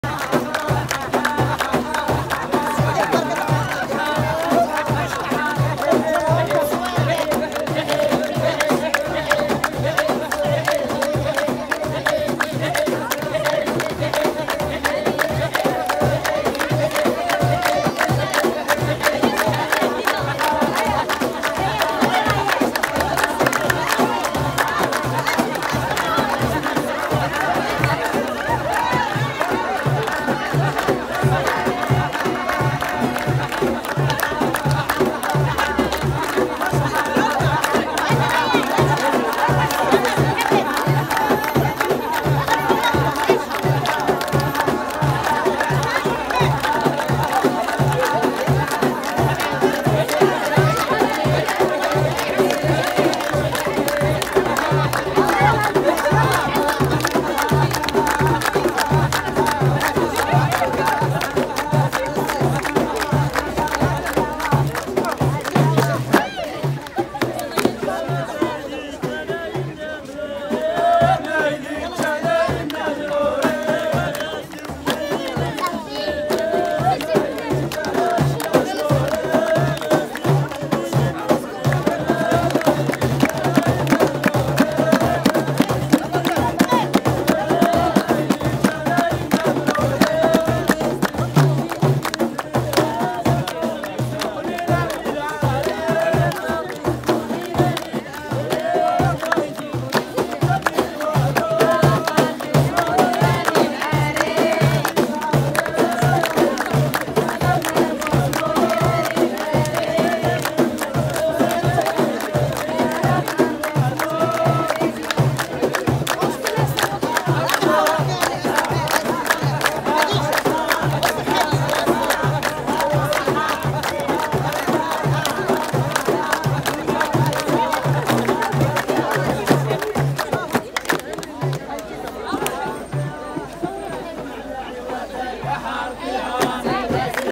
النشاط مع ولاد إفران بلبلوها خلينا من مهرجانات الشواطئ
شباب من مدينة افران يخلقون الحدث في ساحة المدينة حيث تجمع حولهم مجموعة من زوار المدينة و الذين راقهم ما قدمه الشباب من أغاني فلكلورية جميلة تفاعل معها الصغار و الكبار